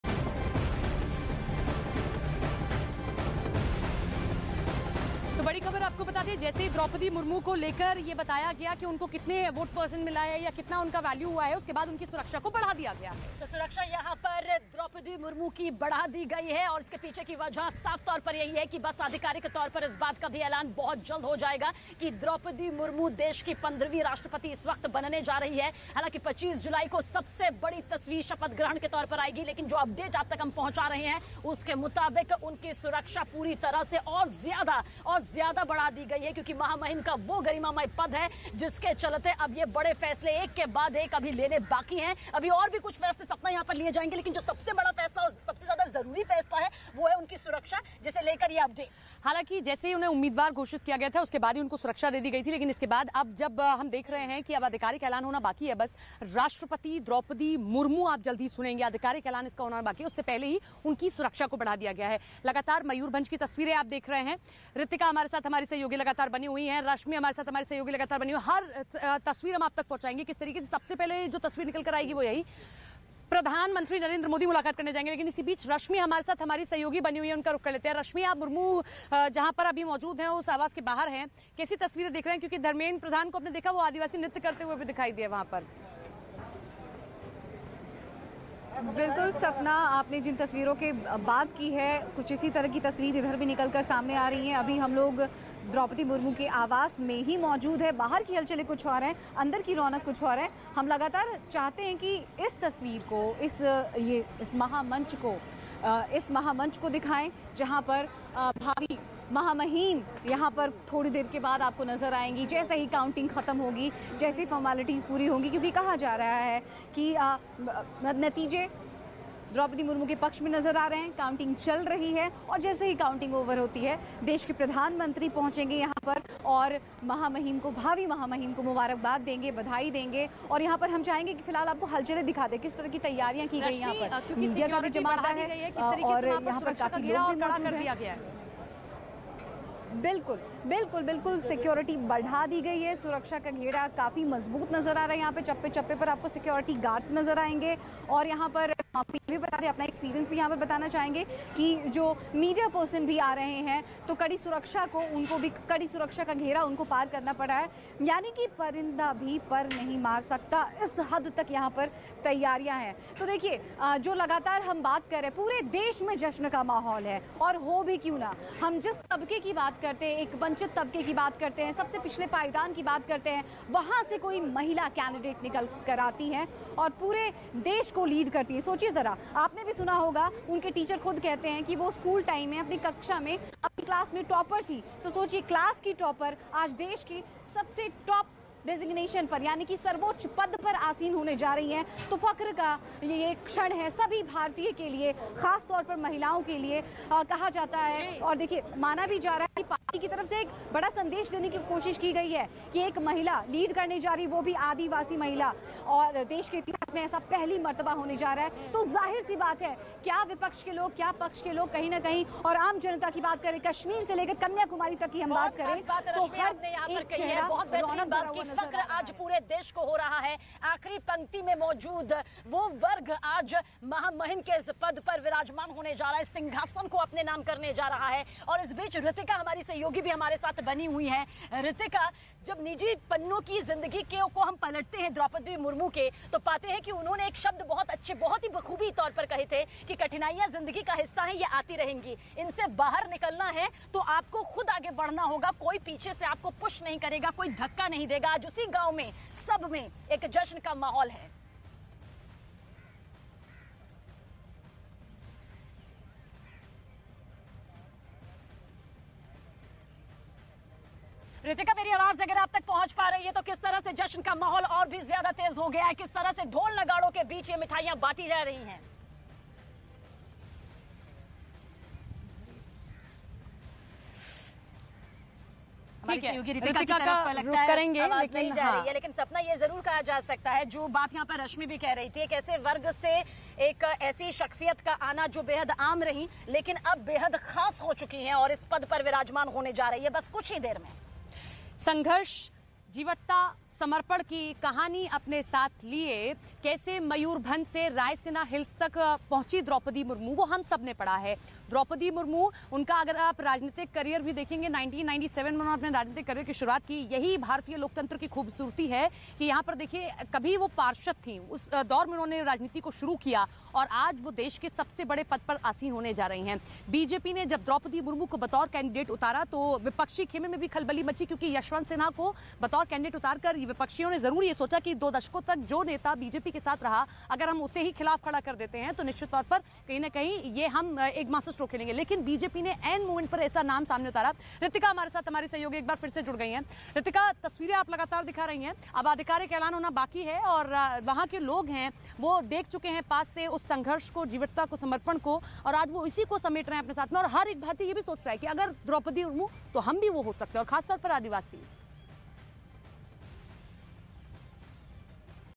Presidential Election 2022: PM Modi आज ही Draupadi Murmu से मिलेंगे, Delhi वाले घर की बढ़ी सुरक्षा
Presidential Election 2022: PM Narendra Modi आज ही Draupadi Murmu से मिलेंगे. करीब 4.30 बजे प्रधानमंत्री नरेंद्र मोदी द्रौपदी मुर्मू के New Delhi स्थित आवास पर मिलने जाएंगे. आवास के बाहर सुरक्षा बढ़ा दी गई है. Delhi Police के जवानों की तैनाती कर दी गई है.